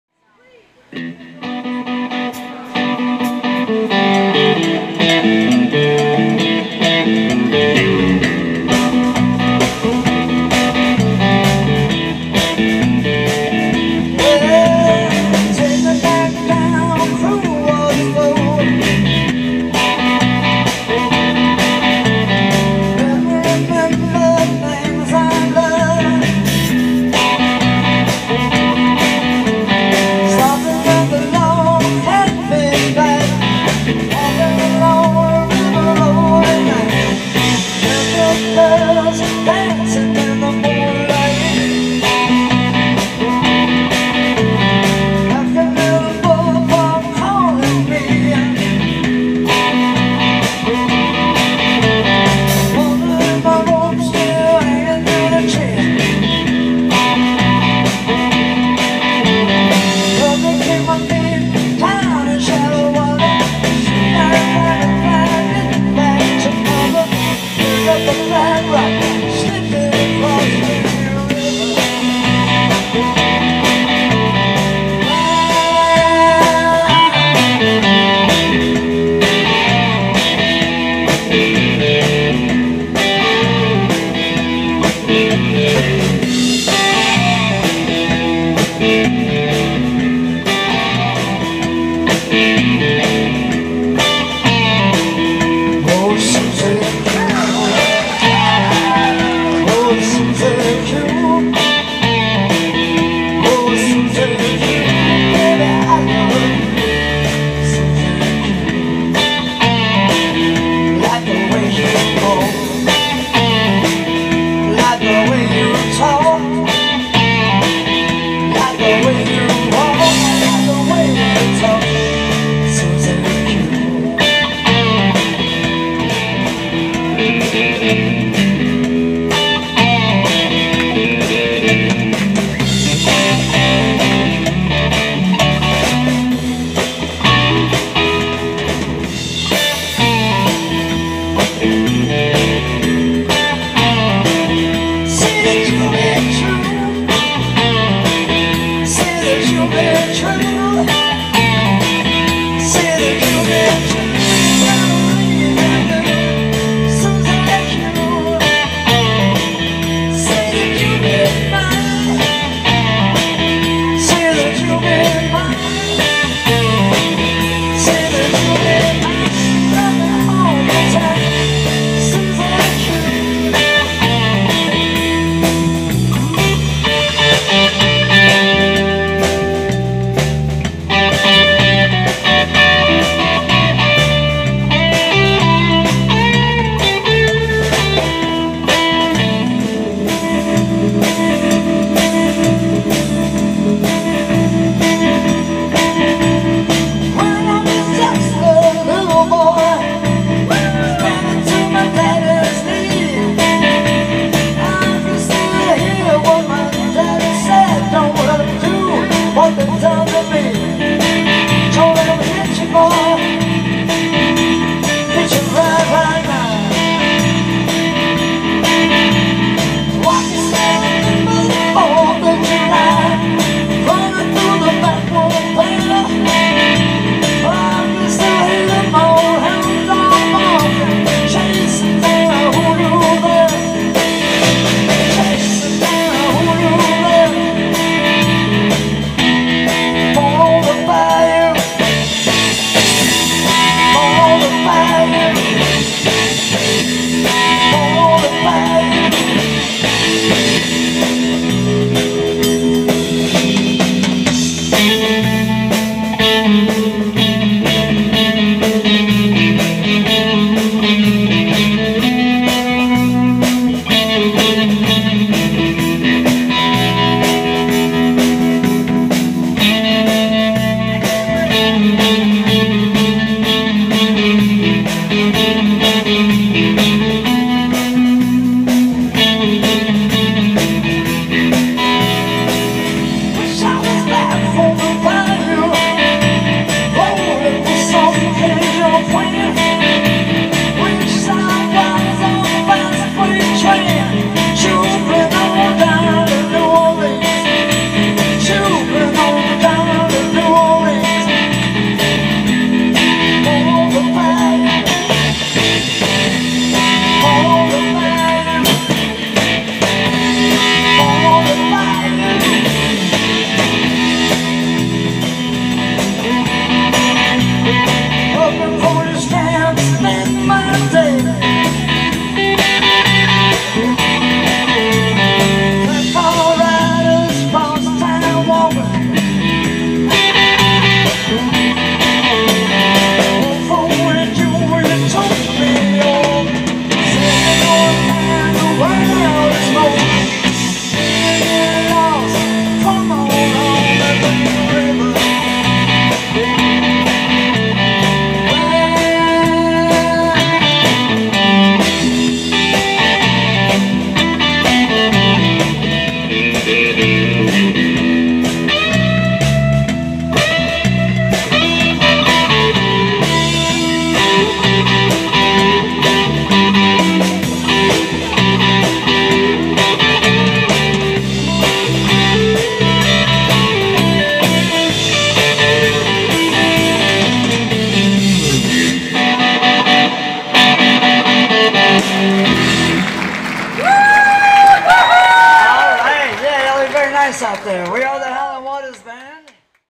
Howlin' Waters Band Live Swamp Rock ("CCR Trilogy")
Guitar, vox - Howlin' Waters; Drums - Georges Barrera; Bass - St Louis Scot Vessell

Creedence Clearwater Revival cover.

Recorded live in Austin, Texas (2023).

 / howlinwaters